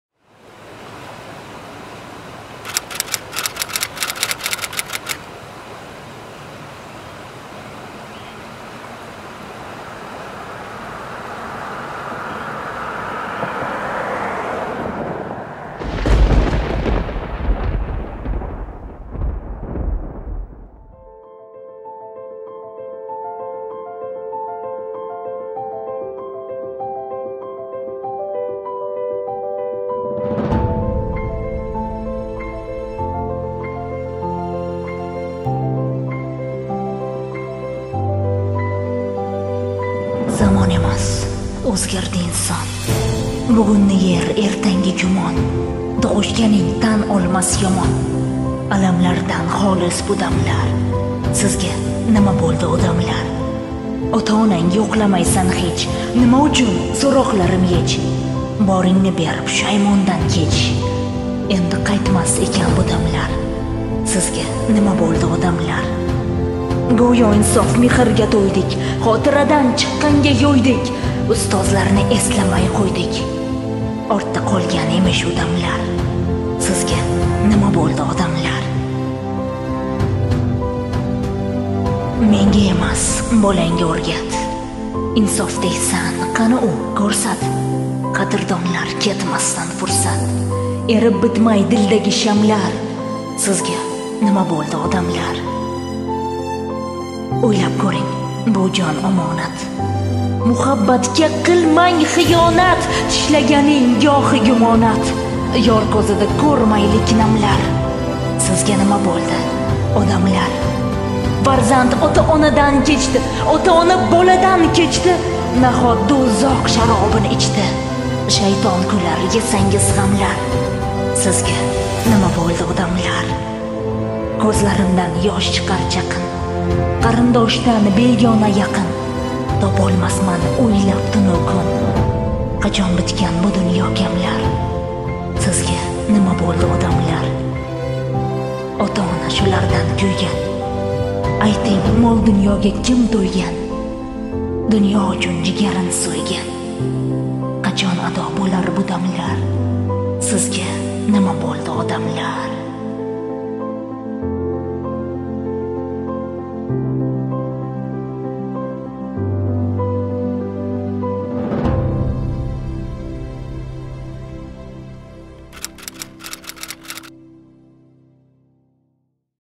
Sizga Nima Bo'ldi Odamlar..(She'r)
"Sizga Nima Bo'ldi Odamlar"- Qisqa Metrajli She'riy Film

Assalomu Alaykum.
Bu video qisqacha she'riy rolikdan iborat. Sizlarga yoqadi degan umiddaman.

She'r Muallifi: Asqar Umr
Ijroda: Dildora Fayzullaeva

E'tiboringiz uchun rahmat, salomat bo'ling.